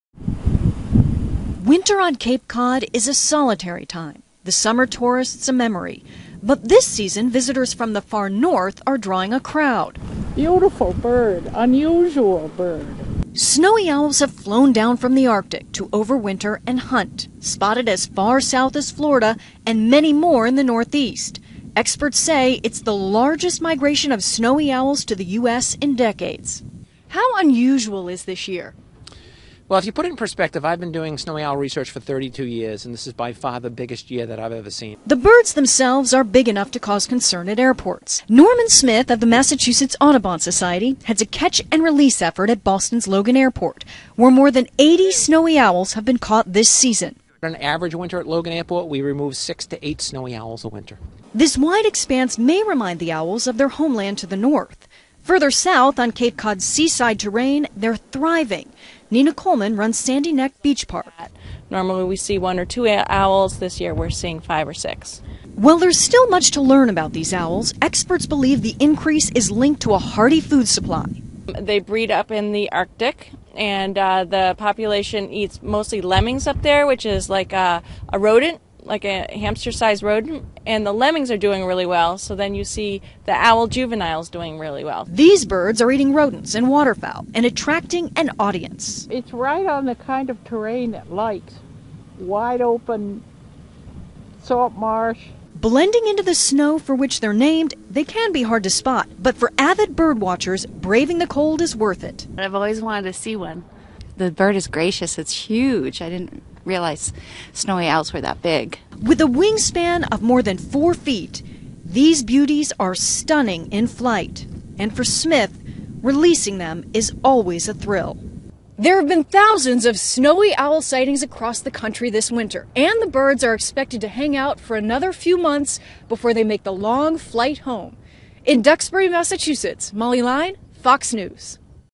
US Experiences Largest Influx of Snowy Owls in Decades
Huge number of birds fly south from the Arctic
Credit:Fox News
Fox News: